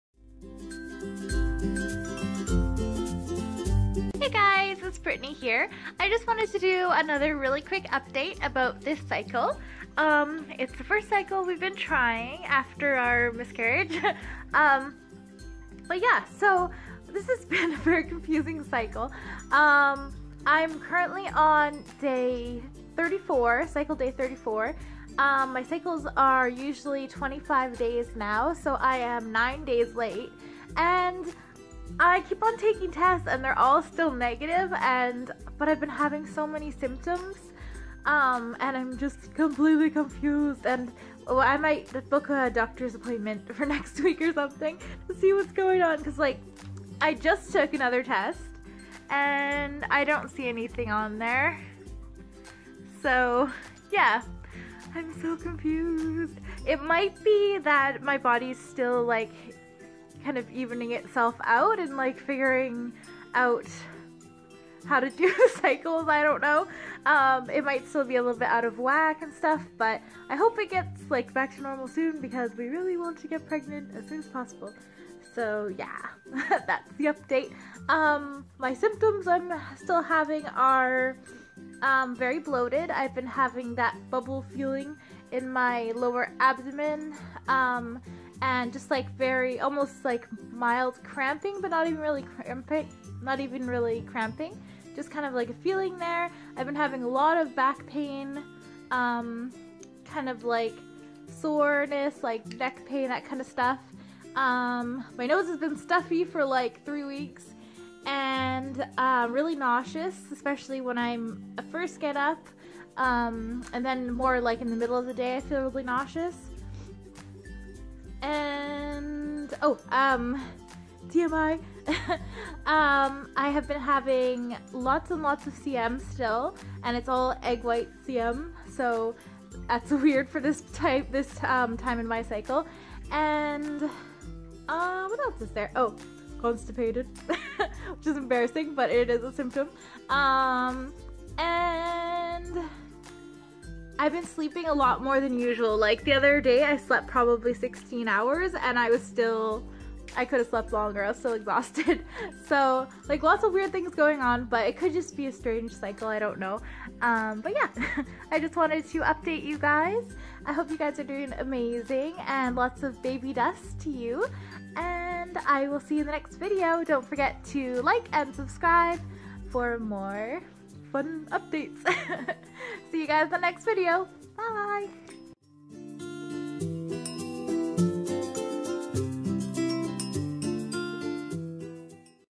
Trying for baby after a miscarriage! First Cycle Update
Love you all! leave us a video suggestion in the comments or just say hi! Remember to add a little love into your life!